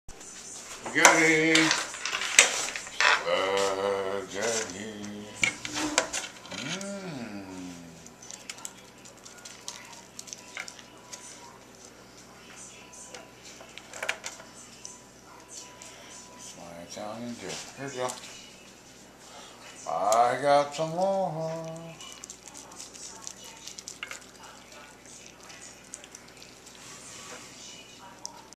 YUM!! Deer eats spaghetti!
Dillie the Deer gets one of her favorite meals: spaghetti!